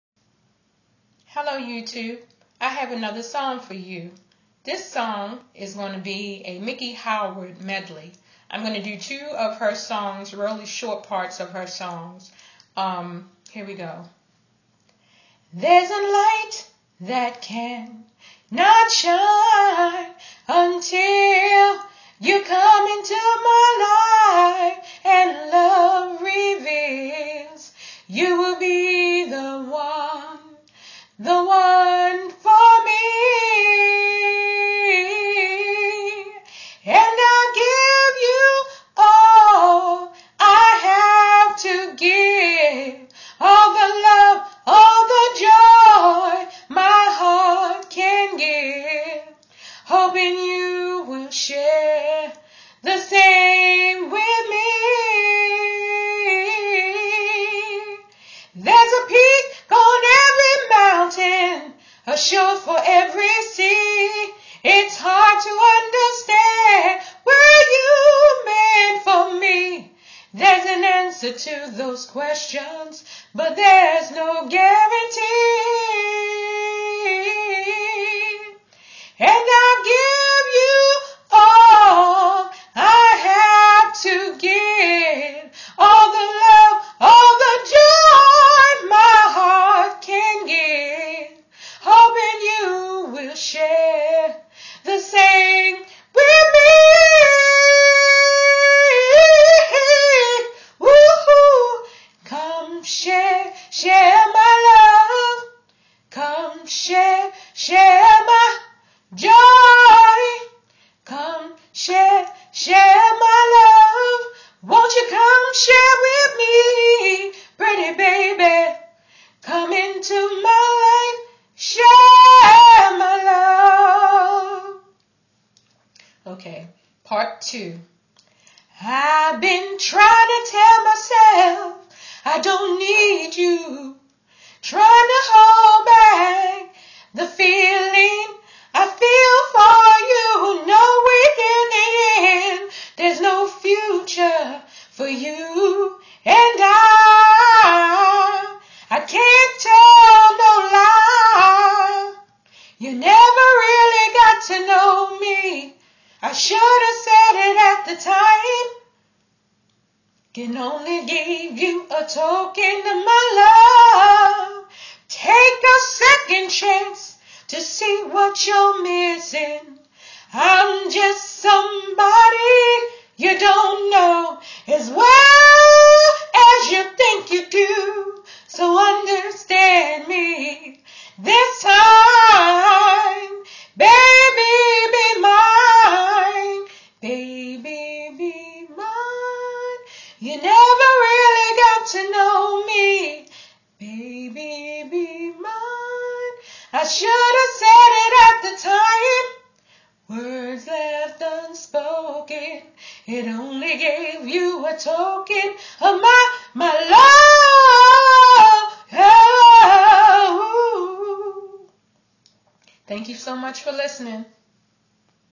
SPP covers Miki Howard Medley
Me singing Miki Howard medley, she is truly an unsung hero. Just want her to know she was and still is appreciated. Much Respect to Miki Howard, come on back sistah!